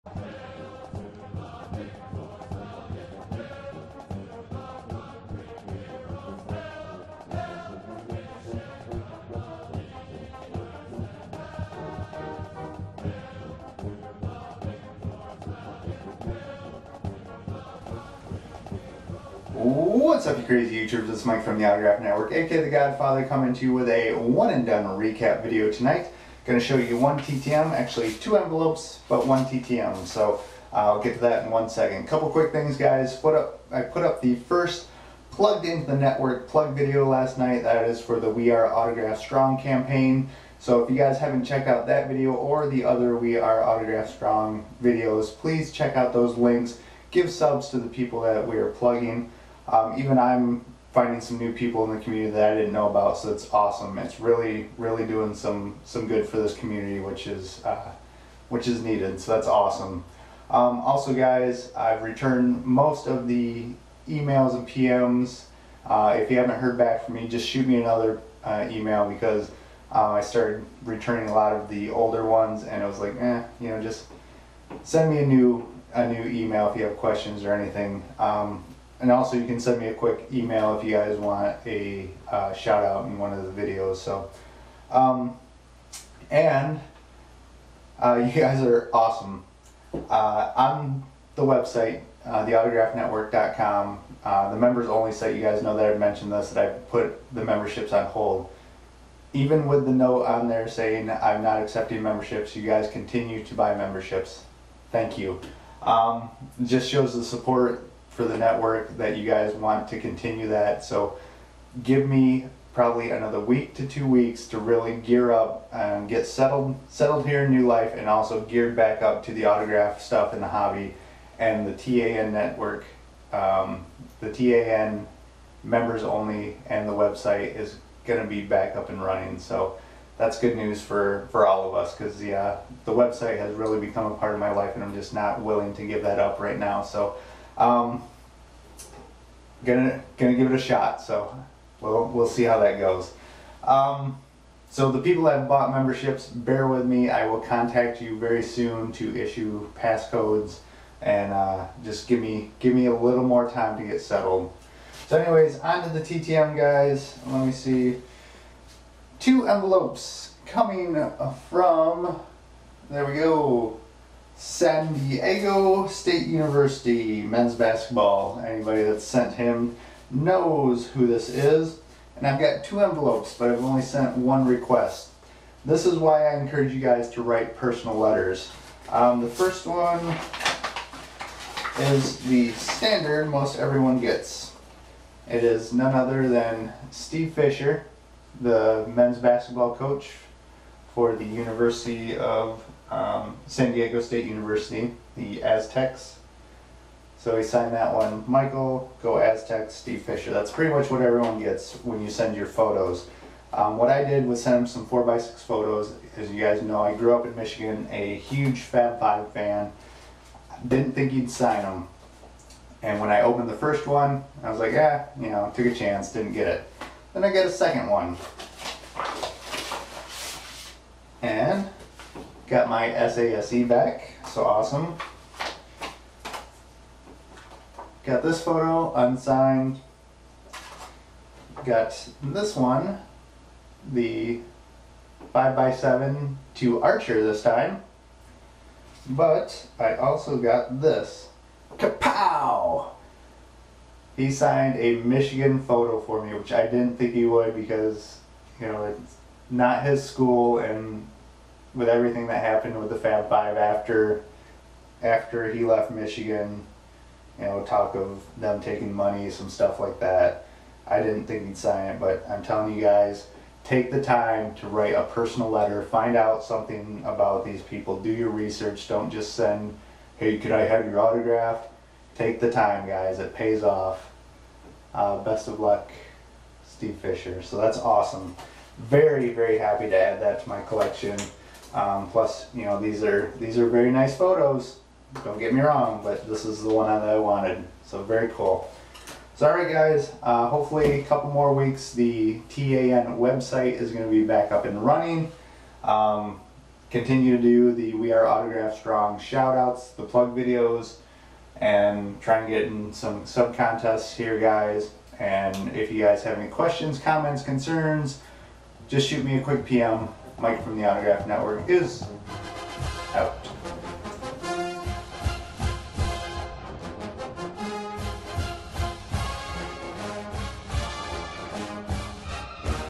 TTM Autographs and More... 10-23-12
I am an autograph Collector; See what goodies showed up in my mail box today. TTM Through The Mail, purchases, treasure hunting, trades , just becauses and other random stuff